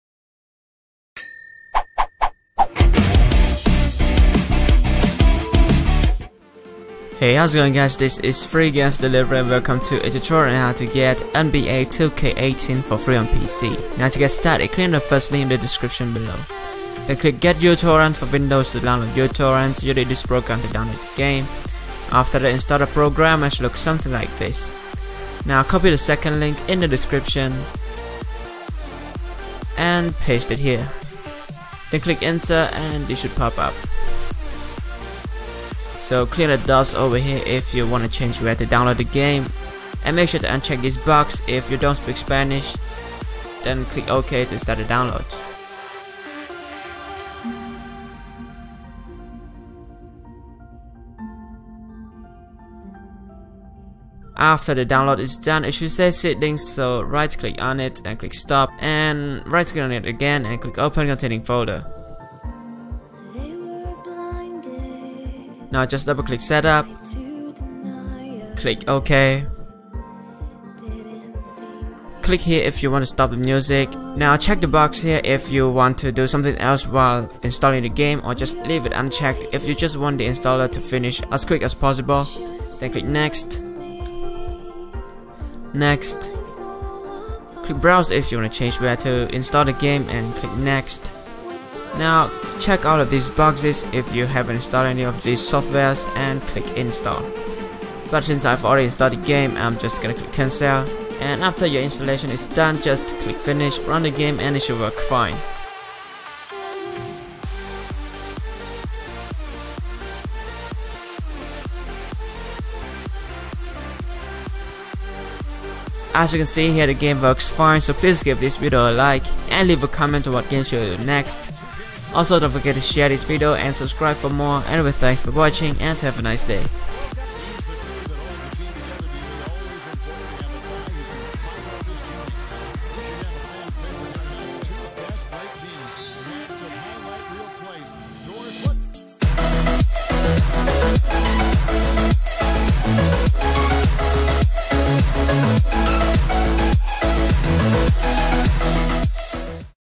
How to get NBA 2K18 for free on PC [Voice Tutorial]
Minimum System Requirements:

CPU: Intel® Core™ i3-530 @ 2.93 GHz / AMD Phenom™ II X4 805 @ 2.50 GHz or better
CPU SPEED: Info
RAM: 4 GB
OS: Windows 7x64 / Windows 8.1x64 / Windows 10x64
VIDEO CARD: NVIDIA® GeForce® GT 450 1GB / ATI® Radeon™ HD 6770 1GB or better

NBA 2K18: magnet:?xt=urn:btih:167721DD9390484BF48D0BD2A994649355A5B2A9